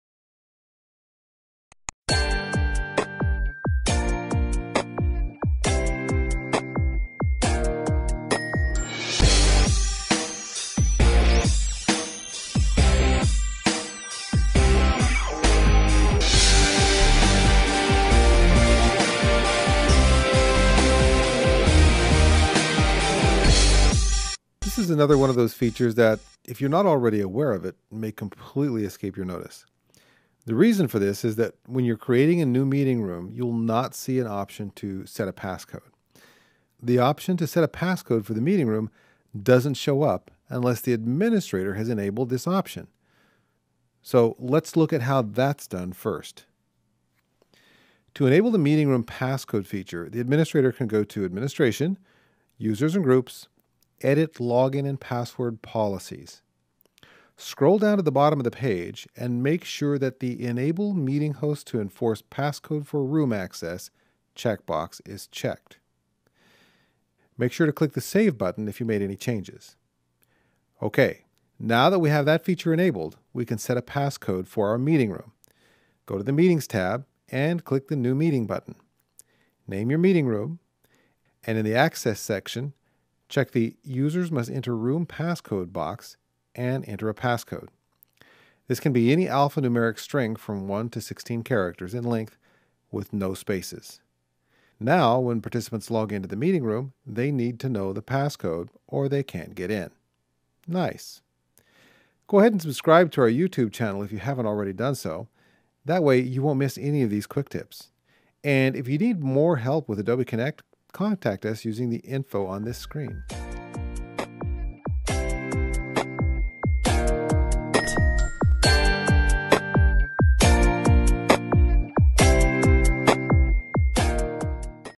How to Add a Passcode to Your Adobe Connect Meeting Room
We show you have to use a passcode with your meeting room.